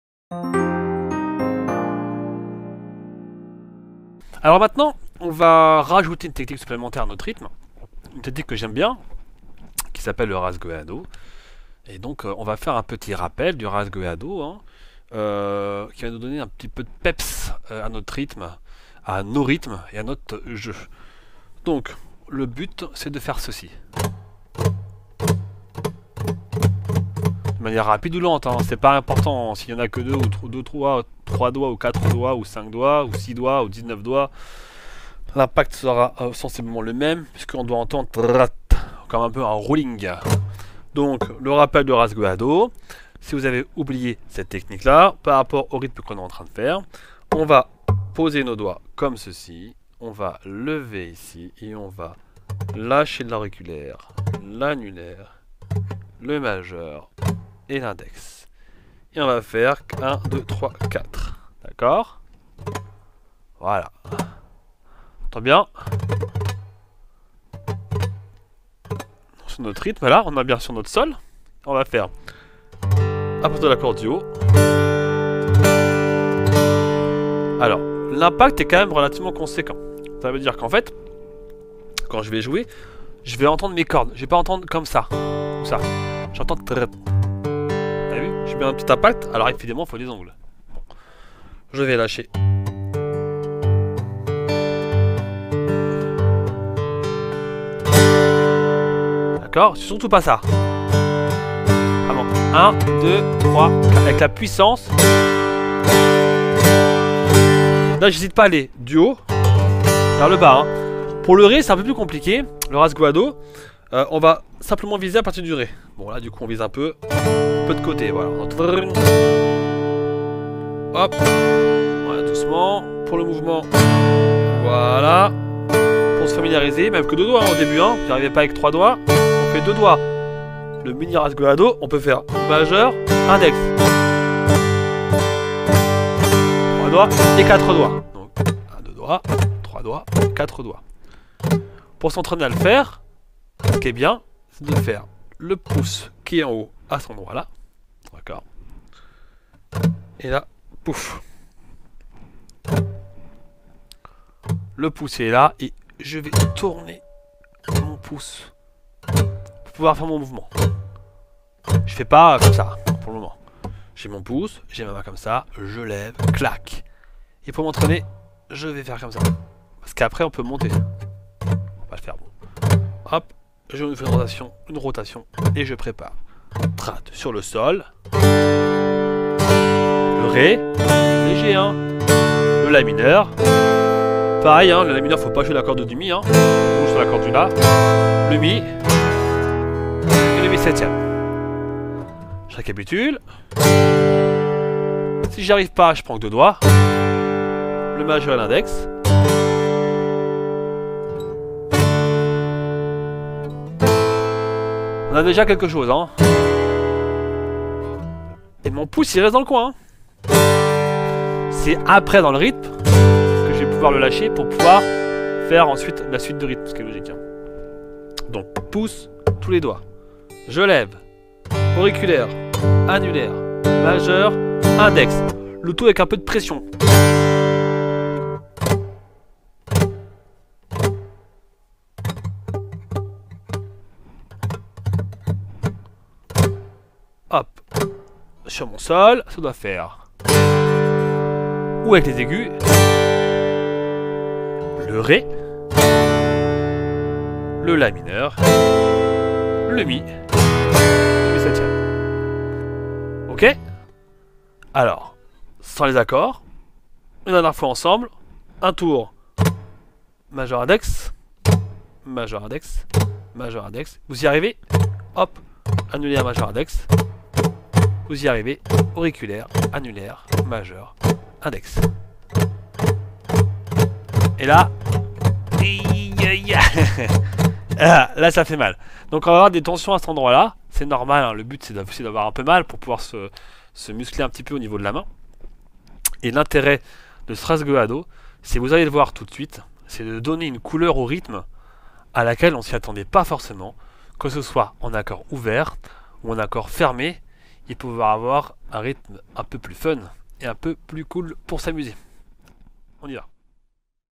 🎸MODÈLES RYTHMIQUES À LA GUITARE : Le Rasgueado - Ruddy MEICHER - imusic-school
🎶 Découvrez le cours de GUITARE de Ruddy MEICHER consacré aux MODÈLES RYTHMIQUES !
🎸 Vous voulez progresser à la guitare ?

Découvrez le plus grand catalogue français de cours et masterclass de guitare en ligne :